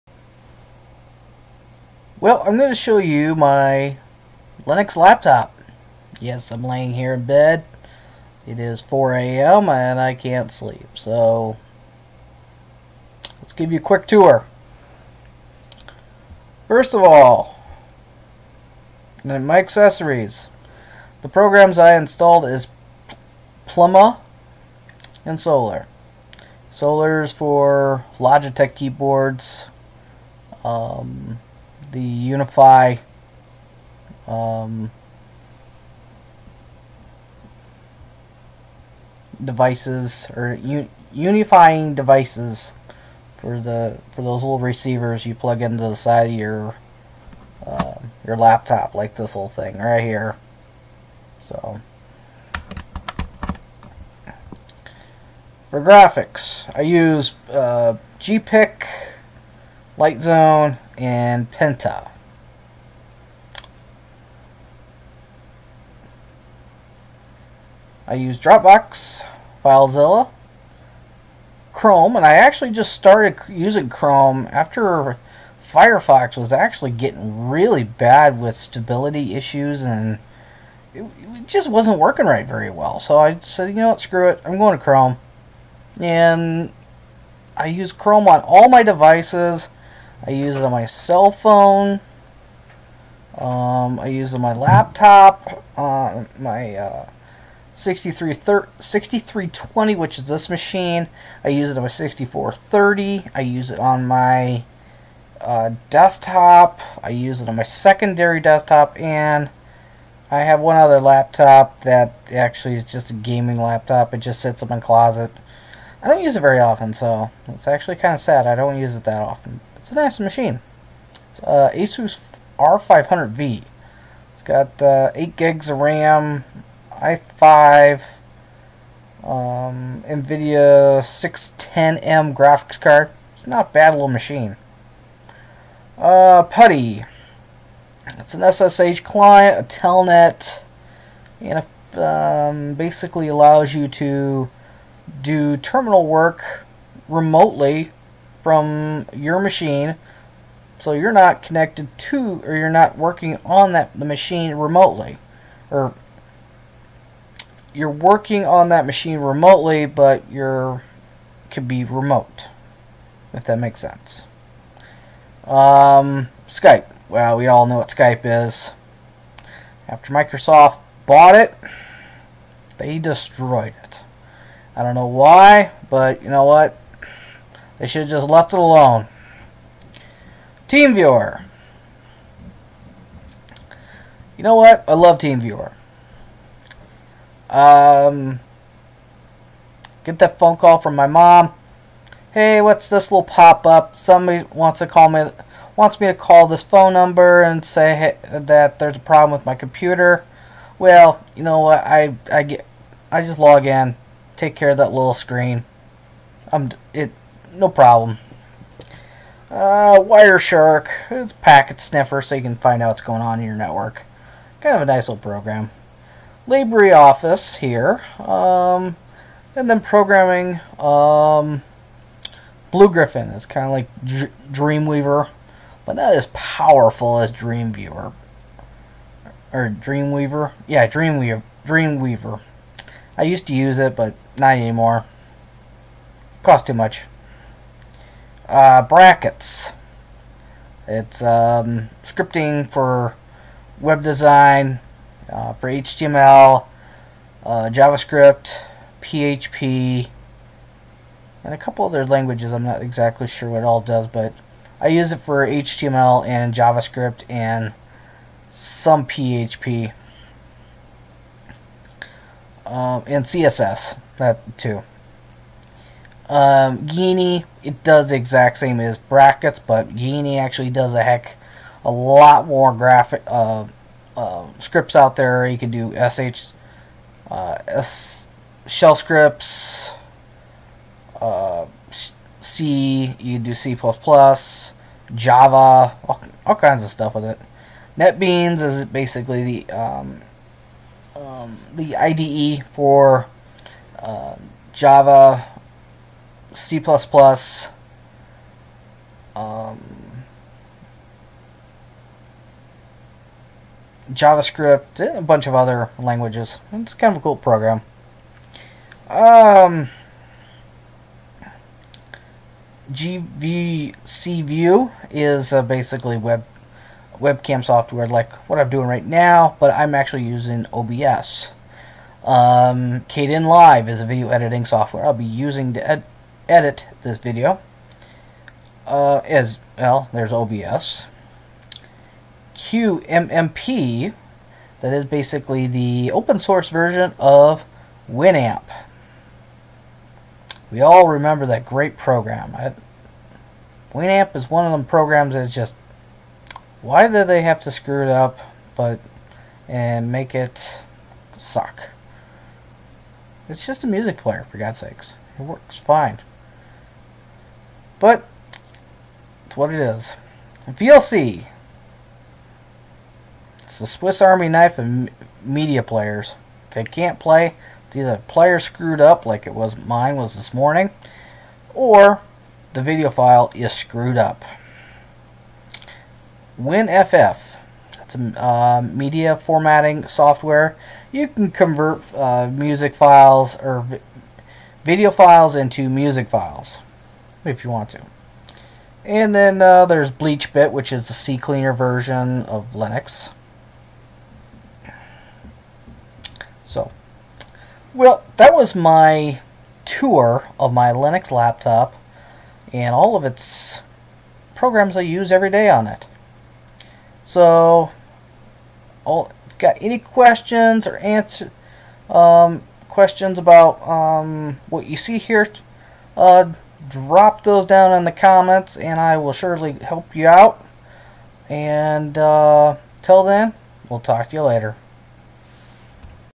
Whats installed my Linux laptop
My specs
Dell 6320
i5 2520m 2.50ghz
8gb Ram
Adata 120gb SSD
Logitech M510